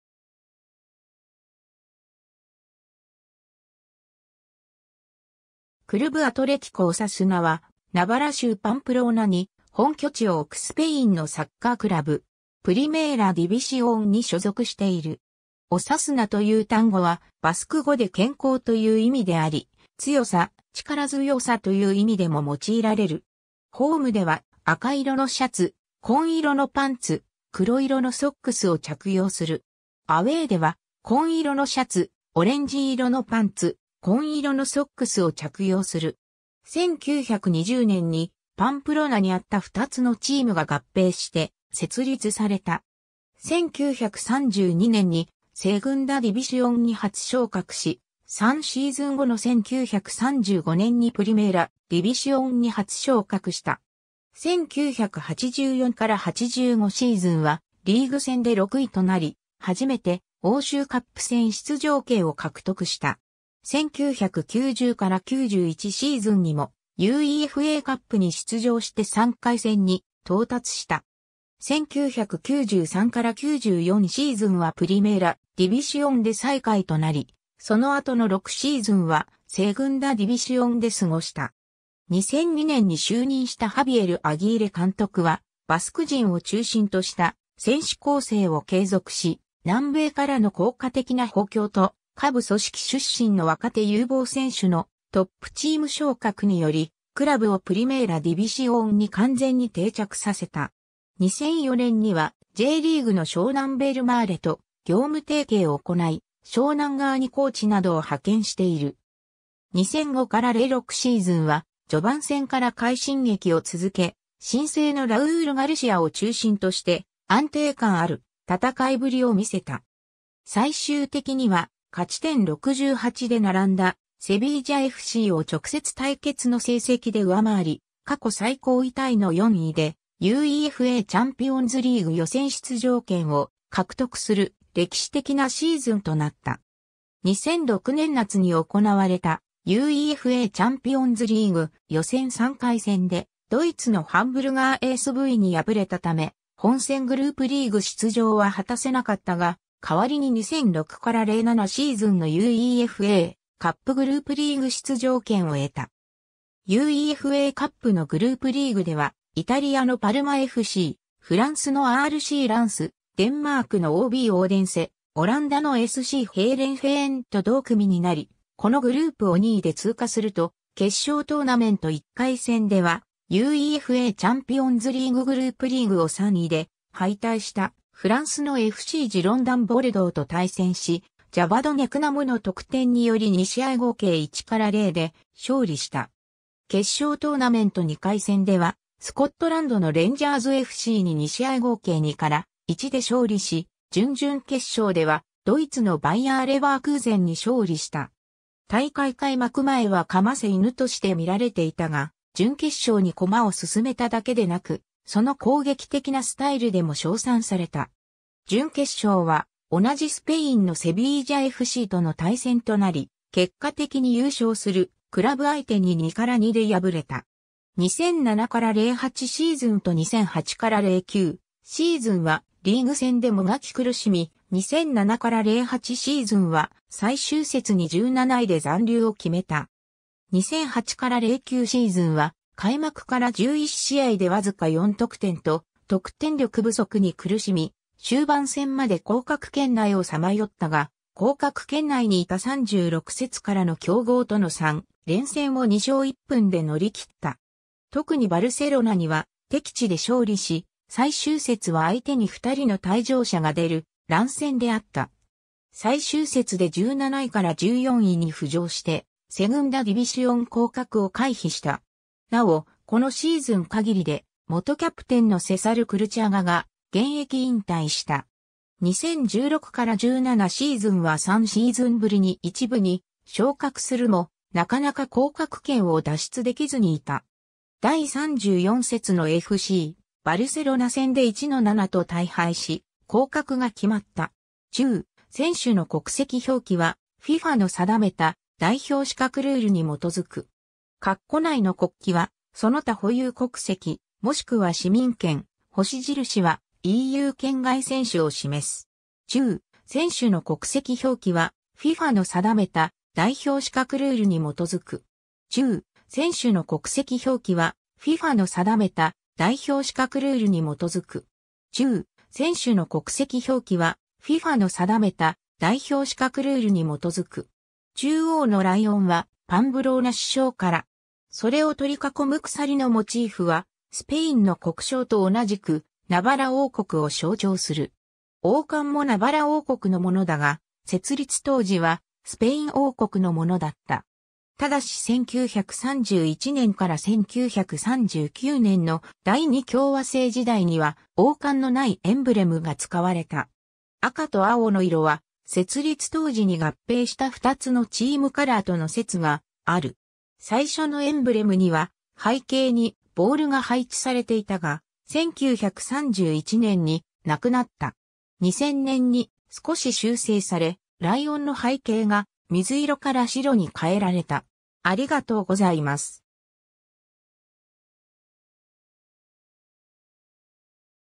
CAオサスナ
クルブ・アトレティコ・オサスナ（スペイン語: Club Atlético Osasuna、スペイン語発音: [ˈkluβ aðˈletiko osaˈsuna]）は、ナバラ州パンプローナに本拠地を置くスペインのサッカークラブ。
プリメーラ・ディビシオンに所属している。
 Osasunaという単語はバスク語で健康(health)という意味であり、強さ/力強さ(strength, vigor)という意味でも用いられる"。
ホームでは赤色のシャツ、紺色のパンツ、黒色のソックスを着用する。
アウェーでは紺色のシャツ、オレンジ色のパンツ、紺色のソックスを着用する。
 1920年にパンプローナにあった2つのチームが合併して設立された。
1932年にセグンダ・ディビシオン（2部）に初昇格し、3シーズン後の1935年にプリメーラ・ディビシオン（1部）に初昇格した。
1984-85シーズンはリーグ戦で6位となり、初めて欧州カップ戦（UEFAカップ）出場権を獲得した。
1990-91シーズンにもUEFAカップに出場して3回戦に到達した。
1993-94シーズンはプリメーラ・ディビシオンで最下位となり、その後の6シーズンはセグンダ・ディビシオンで過ごした。
 2002年に就任したハビエル・アギーレ監督はバスク人を中心とした選手構成を継続し、南米からの効果的な補強と下部組織出身の若手有望選手のトップチーム昇格により、クラブをプリメーラ・ディビシオンに完全に定着させた。
2004年にはJリーグの湘南ベルマーレと業務提携を行い、湘南側にコーチなどを派遣している。
2005-06シーズンは序盤戦から快進撃を続け、新星のラウール・ガルシアを中心として安定感ある戦いぶりを見せた。
最終的には勝ち点68で並んだセビージャFCを直接対決の成績（オサスナの2勝）で上回り、過去最高位タイの4位でUEFAチャンピオンズリーグ予選出場権を獲得する歴史的なシーズンとなった。
2006年夏に行われたUEFAチャンピオンズリーグ・予選3回戦でドイツのハンブルガーSVに敗れたため、本選グループリーグ出場は果たせなかったが、代わりに2006-07シーズンのUEFAカップグループリーグ出場権を得た。
UEFAカップのグループリーグではイタリアのパルマFC、フランスのRCランス、デンマークのOBオーデンセ、オランダのSCヘーレンフェーンと同組になり、このグループを2位で通過すると、決勝トーナメント1回戦ではUEFAチャンピオンズリーググループリーグを3位で敗退したフランスのFCジロンダン・ボルドーと対戦し、ジャバド・ネクナムの得点により2試合合計1-0で勝利した。
決勝トーナメント2回戦ではスコットランドのレンジャーズFCに2試合合計2-1で勝利し、準々決勝ではドイツのバイヤー・レヴァークーゼンに勝利した。
大会開幕前はかませ犬として見られていたが、準決勝に駒を進めただけでなく、その攻撃的なスタイルでも称賛された。
準決勝は同じスペインのセビージャFCとの対戦となり、結果的に優勝するクラブ相手に2-2（アウェーゴール差）で敗れた。
2007-08シーズンと2008-09シーズンはリーグ戦でもがき苦しみ、2007-08シーズンは最終節に17位で残留を決めた。
2008-09シーズンは開幕から11試合でわずか4得点と得点力不足に苦しみ、終盤戦まで降格圏内をさまよったが、降格圏内にいた36節からの強豪との3連戦（セビージャFC戦、FCバルセロナ戦、レアル・マドリード戦）を2勝1分で乗り切った。
特にバルセロナには敵地で勝利し、最終節は相手に2人の退場者が出る乱戦であった。
最終節で17位から14位に浮上してセグンダ・ディビシオン降格を回避した。
なお、このシーズン限りで元キャプテンのセサル・クルチャガが現役引退した。
 2016-17シーズンは3シーズンぶりに1部に昇格するも、なかなか降格圏を脱出できずにいた。
第34節のFCバルセロナ戦で1-7と大敗し、降格が決まった。
 ※括弧内の国旗はその他保有国籍、もしくは市民権、星印はEU圏外選手を示す。
 中央のライオンはパンブローナ市章から。
それを取り囲む鎖のモチー...